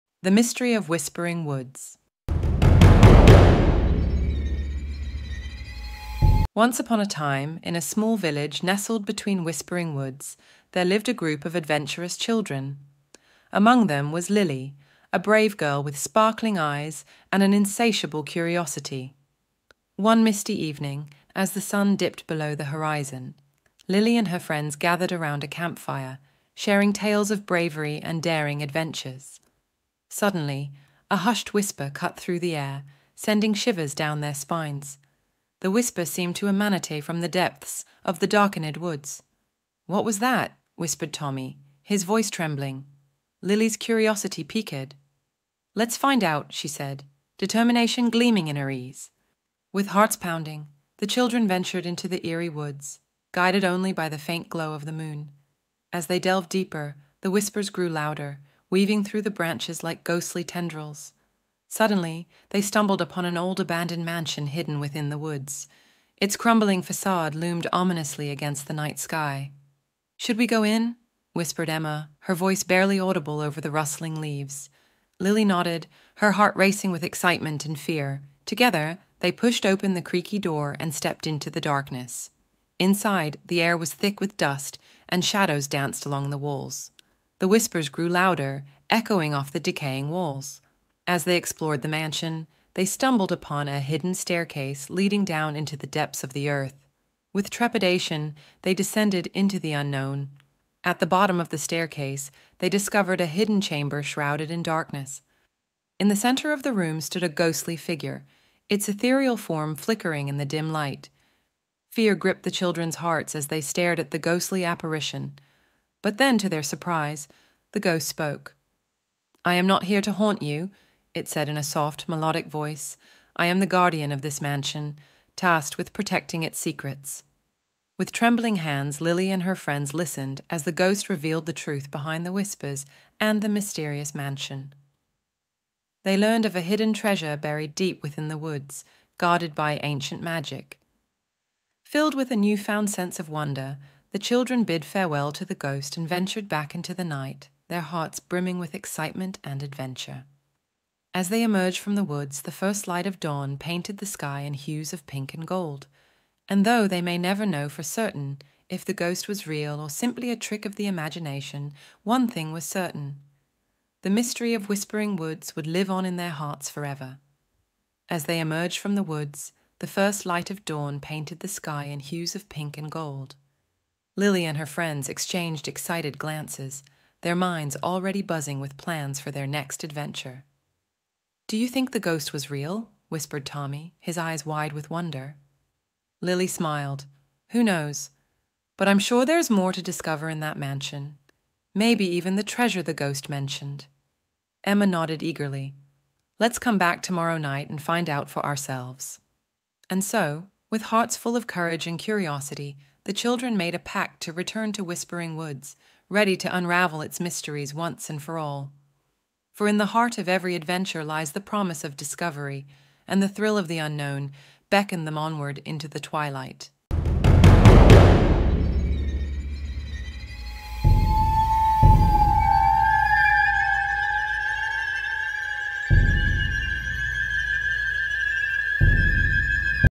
Ghost Story l The Mystery of Whispering Woods l Azhar Niaz
Once upon a time, in a small village nestled between whispering woods, there lived a group of adventurous children. Among them was Lily, a brave girl with sparkling eyes and an insatiable curiosity.